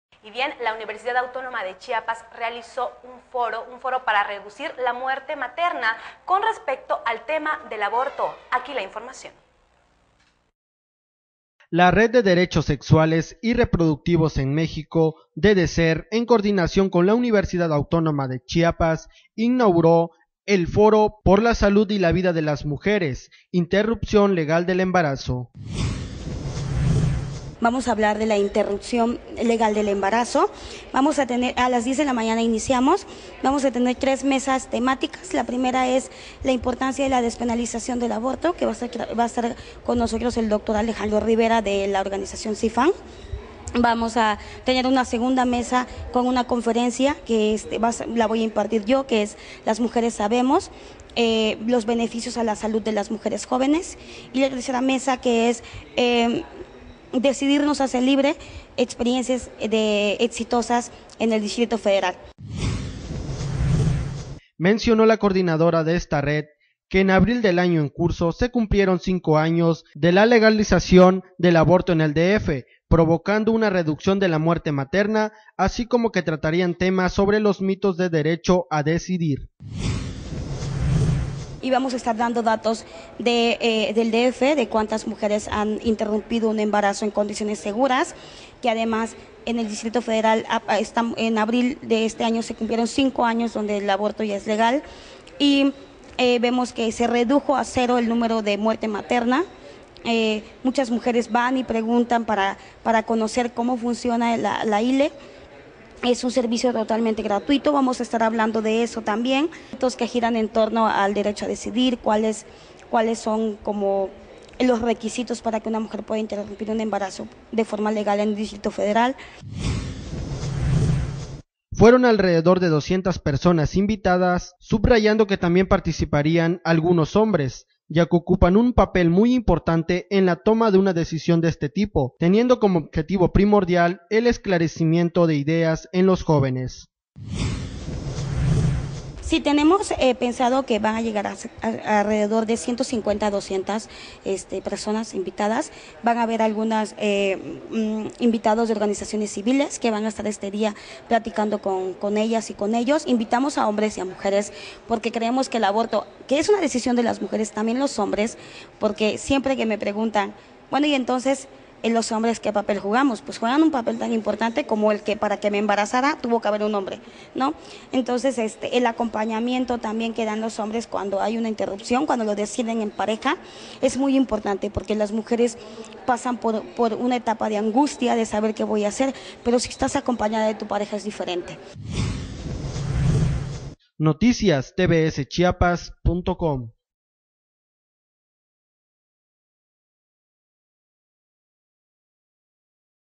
TVS Chiapas.- Foro para reducir muerte materna Organizado por la UNACH, Tuxtla Gutiérrez
- Foro para reducir muerte materna Organizado por la UNACH, Tuxtla Gutiérrez